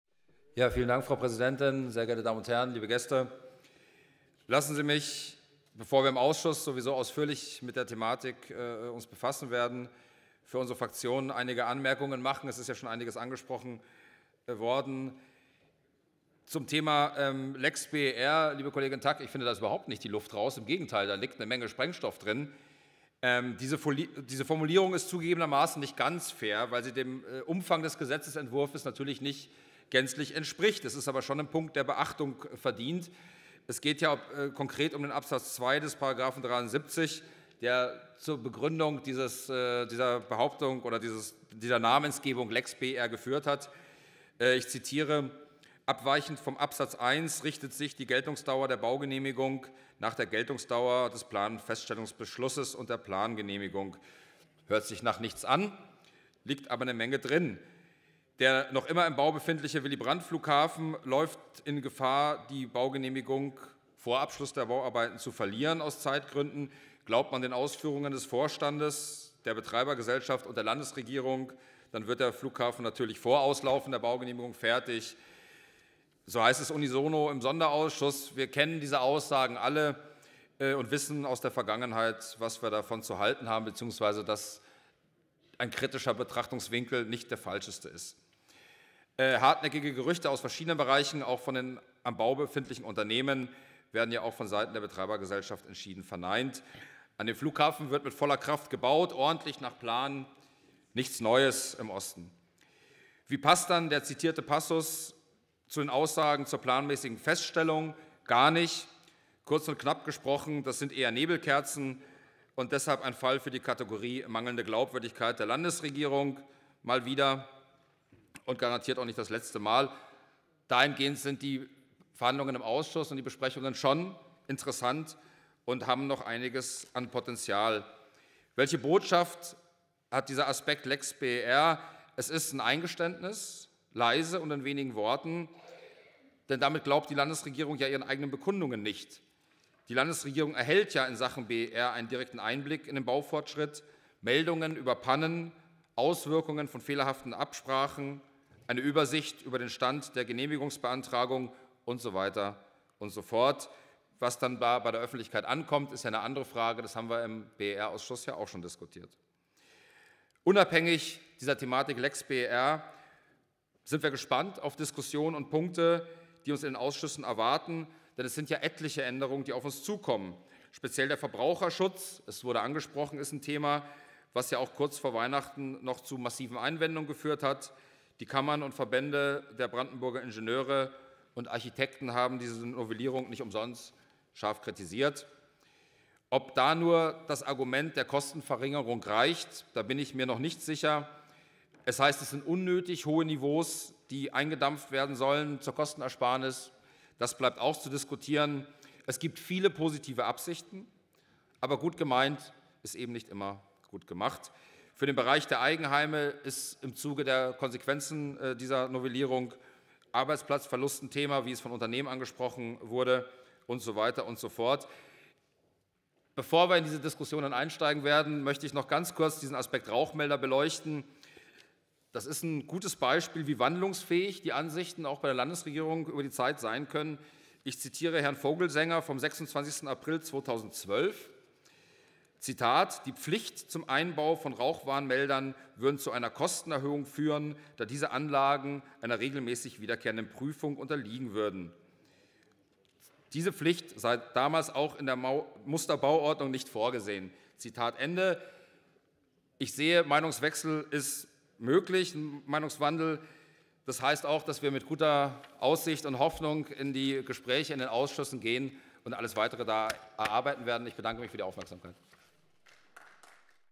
Andreas Kalbitz zur Brandenburgischen Bauordnung
22. Plenarsitzung des Brandenburgischen Landtages

Andreas Kalbitz: "An Frau Tack von den Linken: Ich finde beim Thema BER ist noch lange nicht die Luft raus. Im Gegenteil: Da ist eine Menge Sprengstoff drin!"